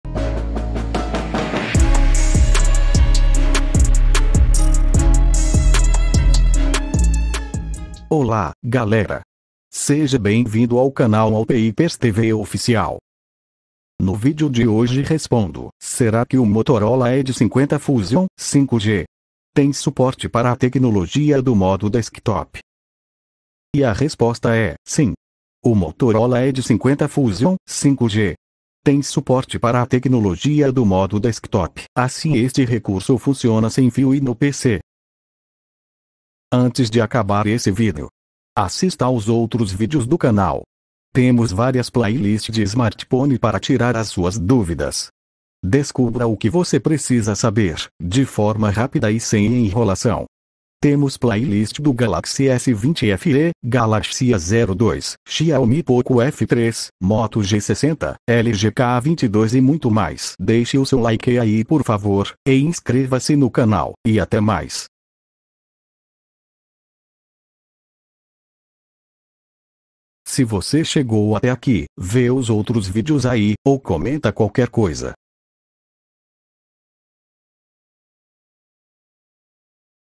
🔴 O MOTOROLA EDGE 50 FUSION 5G TEM MODO DESKTOP?
No vídeo de hoje respondo se o Motorola Edge 50 Fusion 5G tem suporte para a tecnologia do modo desktop, e a resposta é sim. O Motorola Edge 50 Fusion 5G tem suporte para a tecnologia do modo desktop, assim este recurso funciona sem fio e no PC.